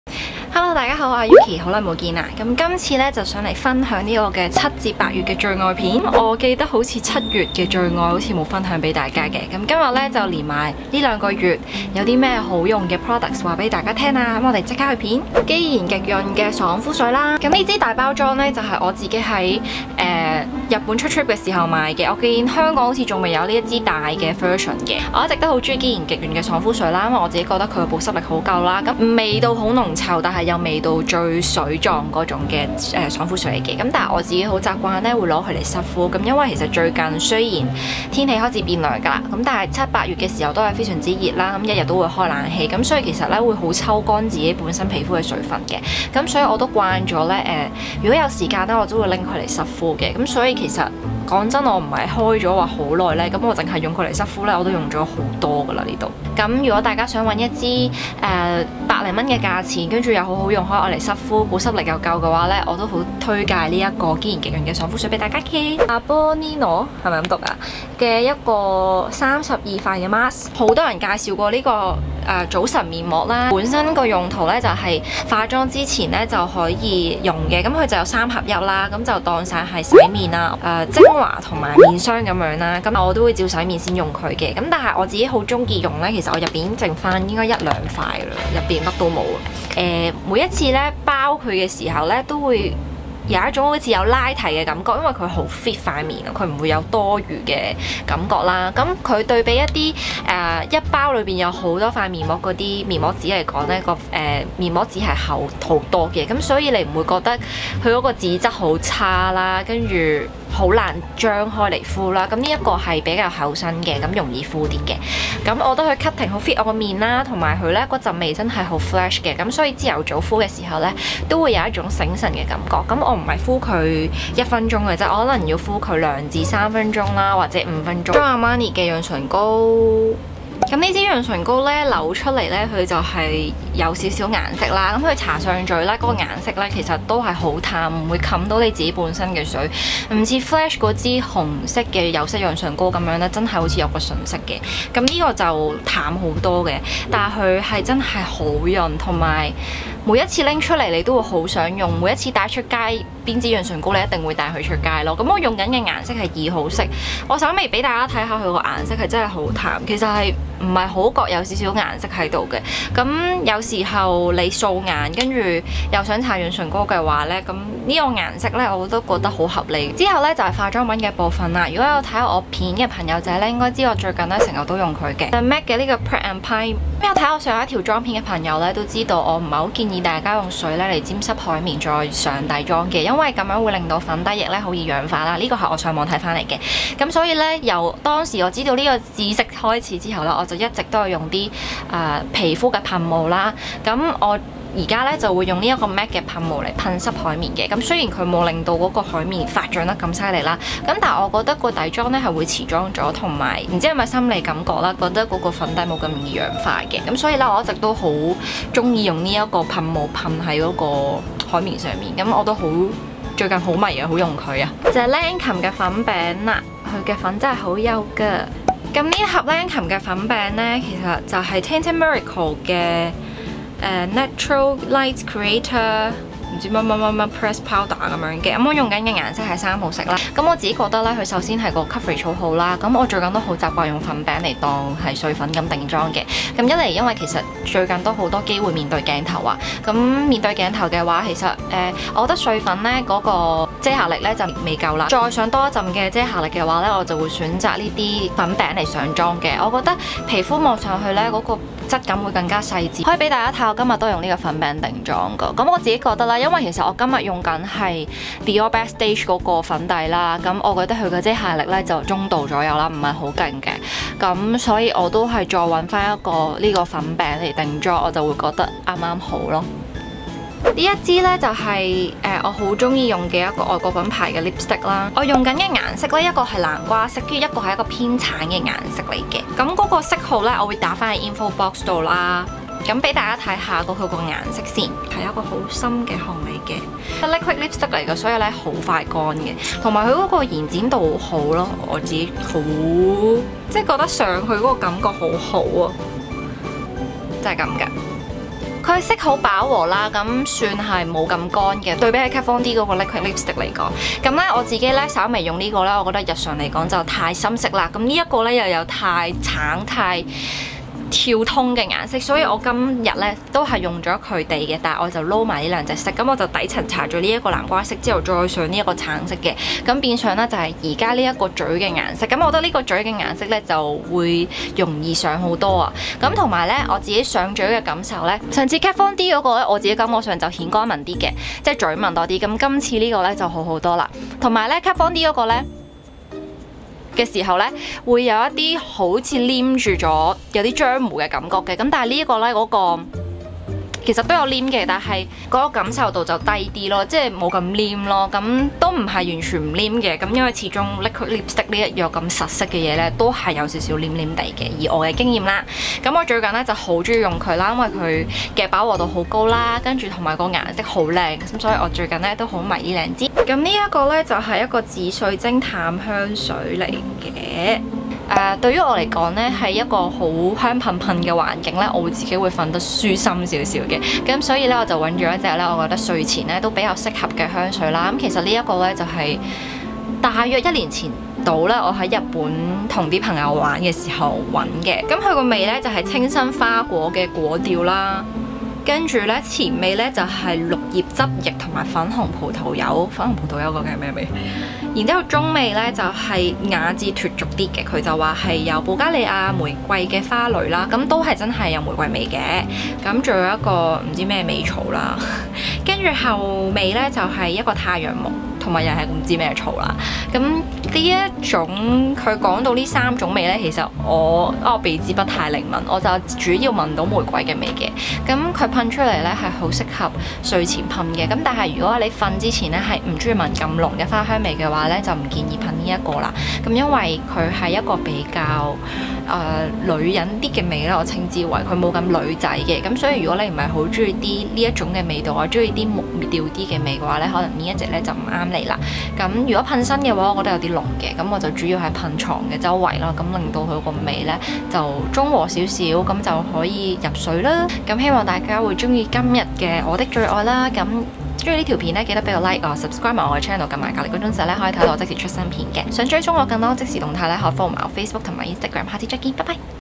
七至八月的愛用品：CP值高面膜、粉餅、唇膏通通有 Recently favourite
前陣子出trip 後發燒了，所以影片就像我的航班一樣延誤了～～～
產品資訊再更新吧～～